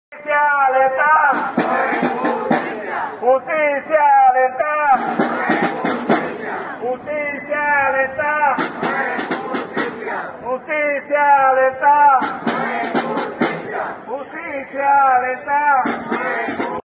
Porque si la justicia no es rápida, no es justicia.
Desde CGT hemos protestado ante las Sedes Judiciales Andaluzas, ante la indefensión que se nos está imponiendo, al no ponerse remedio a los largos tiempos que están teniendo lugar en los procesos judiciales. Y de los cuales las patronales y empresas se están aprovechando para someter a los trabajadores a sus abusos, a sabiendas de que les compensa.